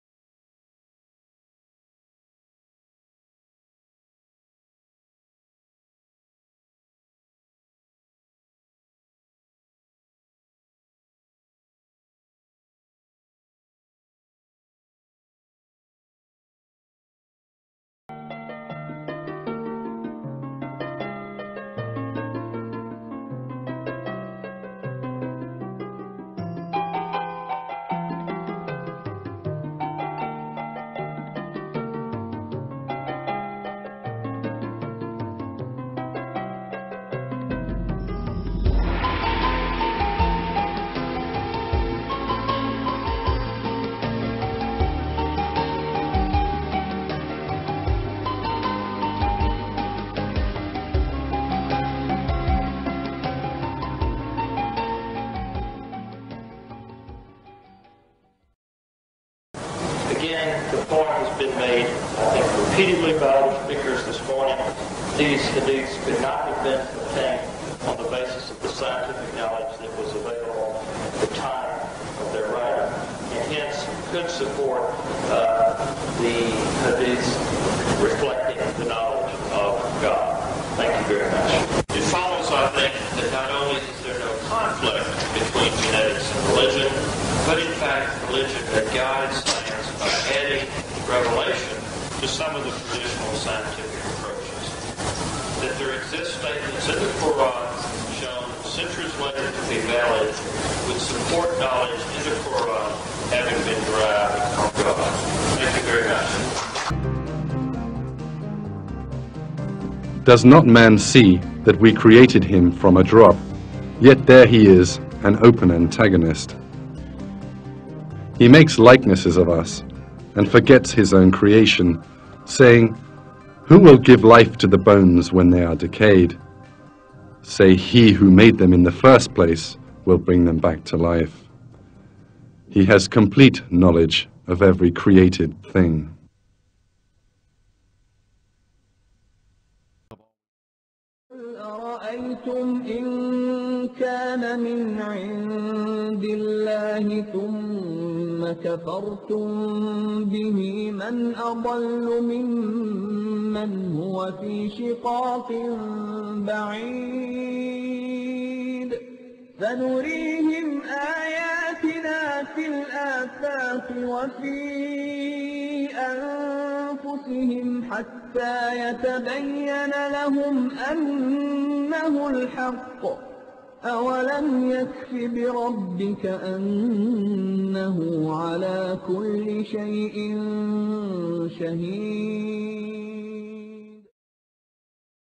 Professor Joe Simpson testifying about Quran descriptions on Genetics
For full books, videos and documentaries on Islam, science and the truth of creation, go to the links